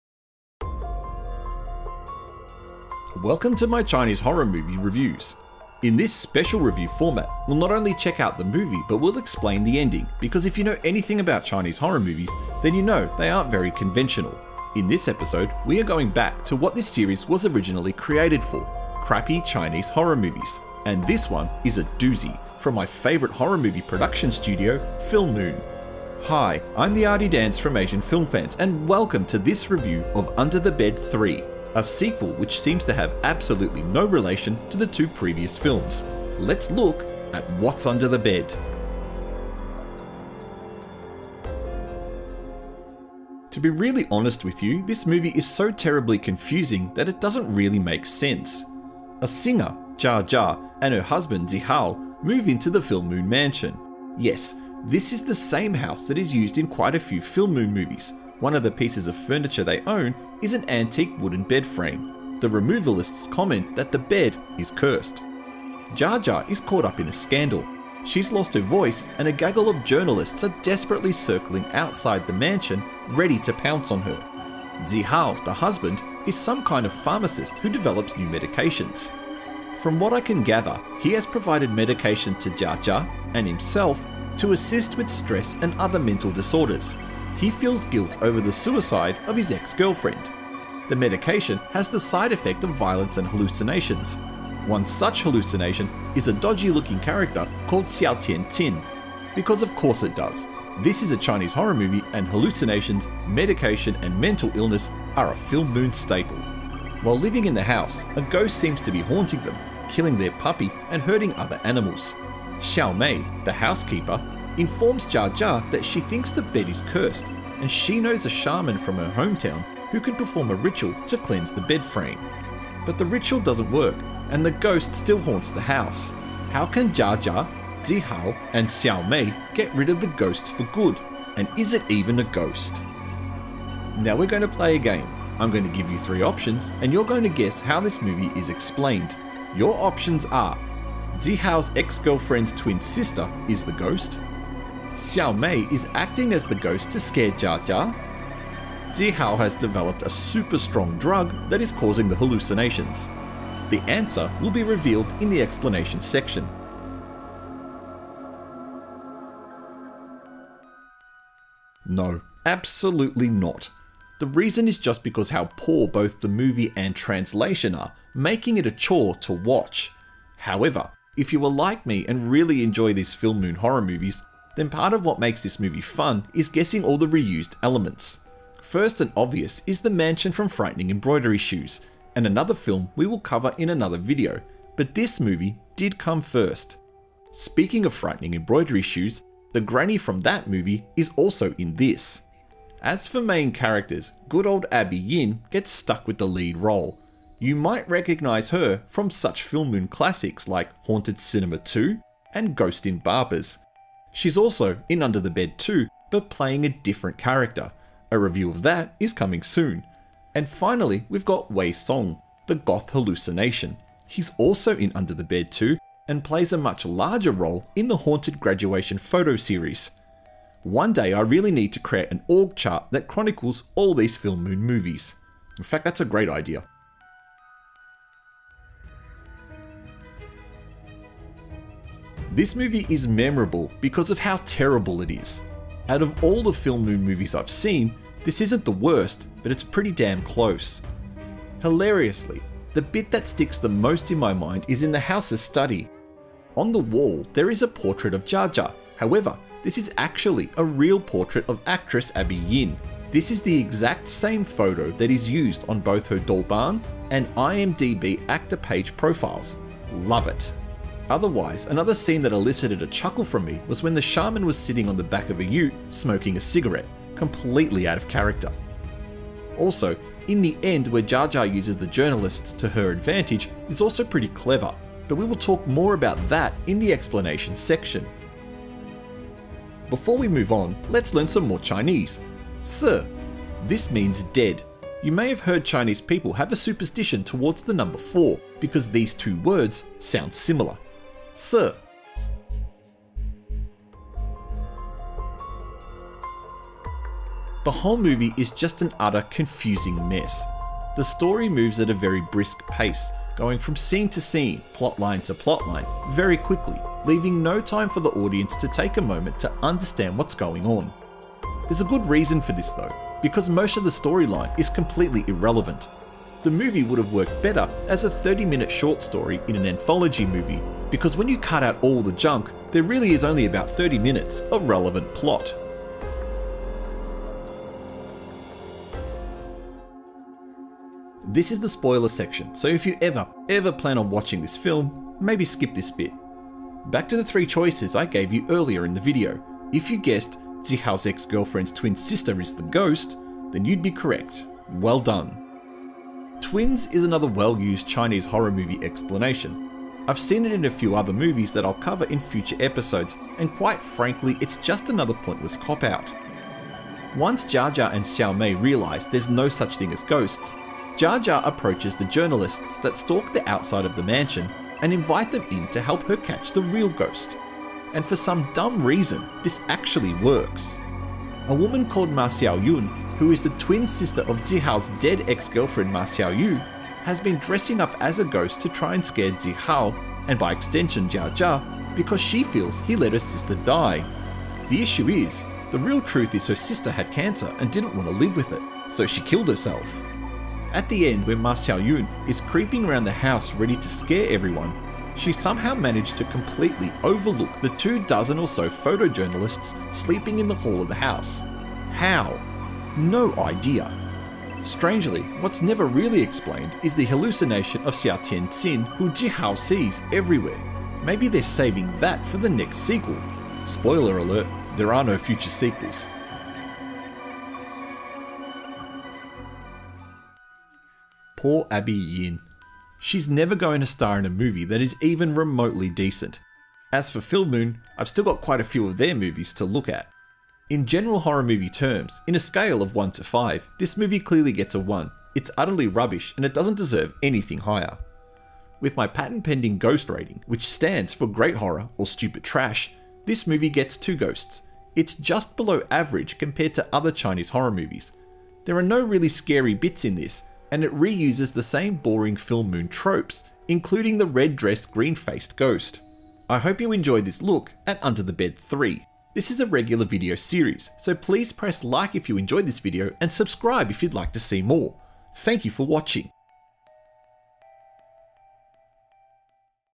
Under The Bed 3 - What's Creeping Under YOUR Bed? | Chinese Horror Review #005 床下有人3
Welcome to my Chinese horror movies reviews - you'll get a review AND an explanation of the ending. Because these movies are so bad you'll need it!

In this episode, it's the Filmoon classic Under The Bed 3!

Genre Horror, somewhat
Director Yongfeng Shang
Starring Abby Yin, Wenxuan Jiang
Original Title 床下有人3